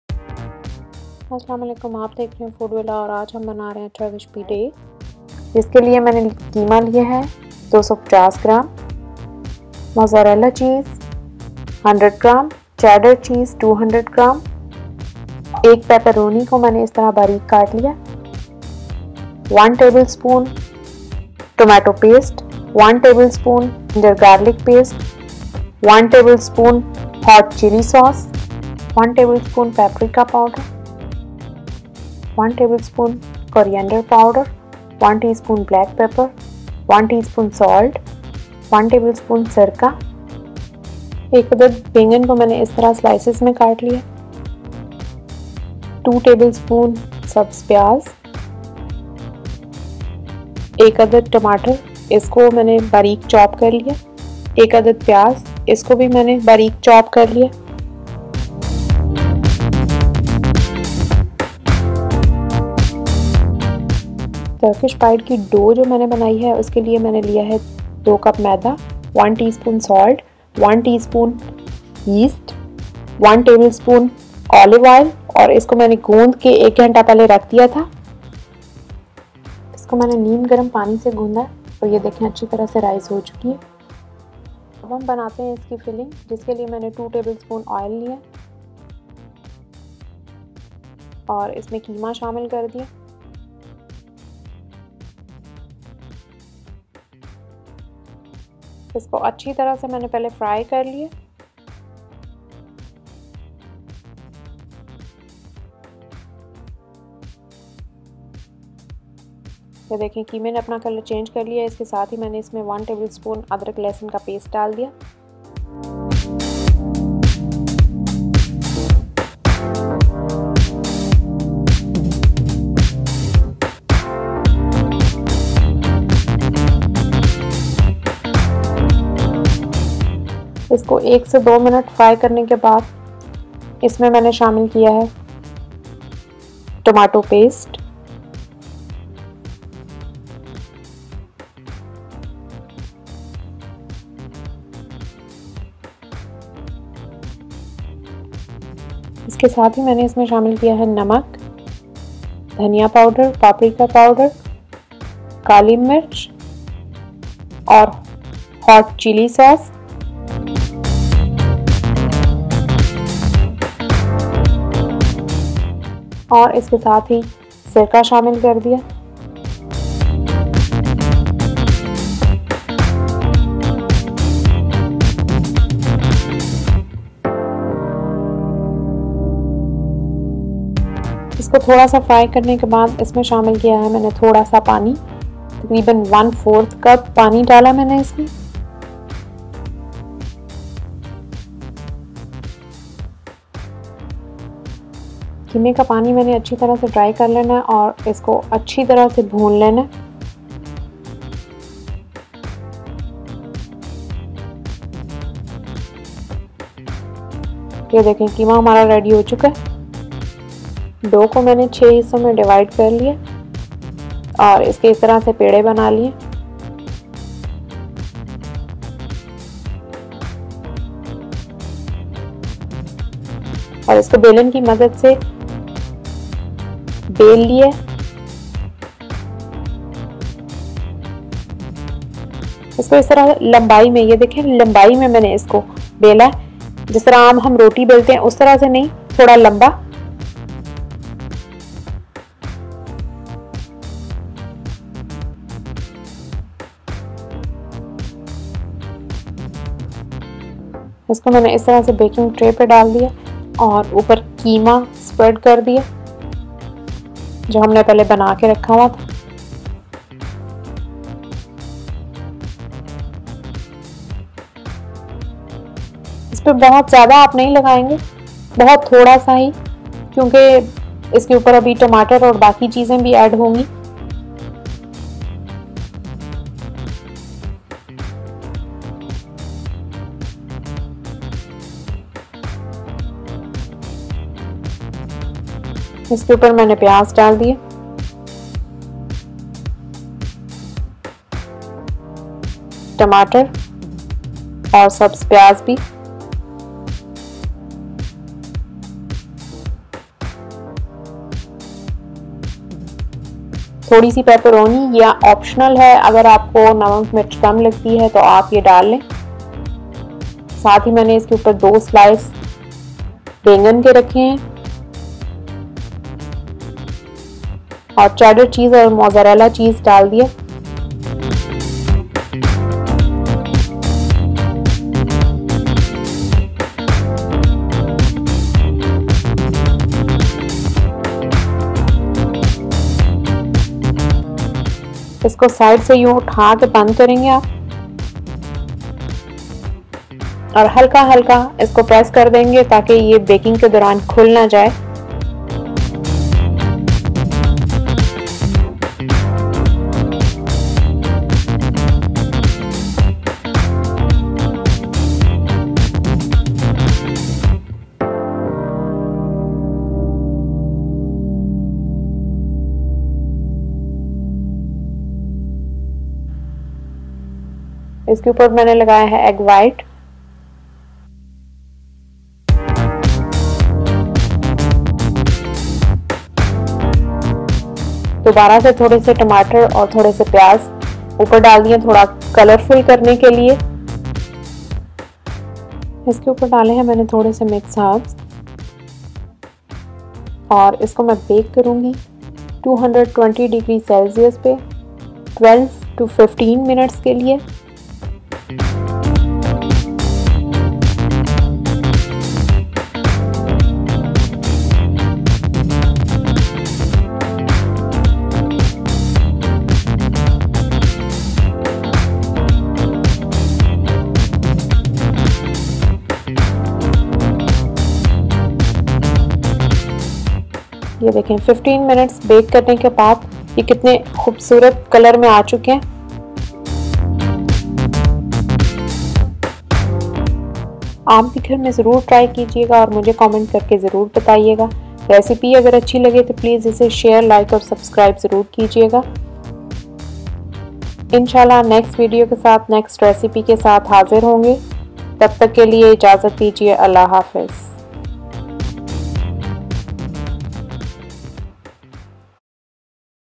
Turkish Pide how to make
Shezwan Chatney
Turkish Pide
Ingredients:
Dough:
luke warm water as required

Filling:
Mince Mutton /Beef  250g
Egg plant 1 sliced
Green Onion chopped 2 tbsp
Paprica powder 1Tbsp
Hot chili sauce 1 Tbsp
Peperoni optional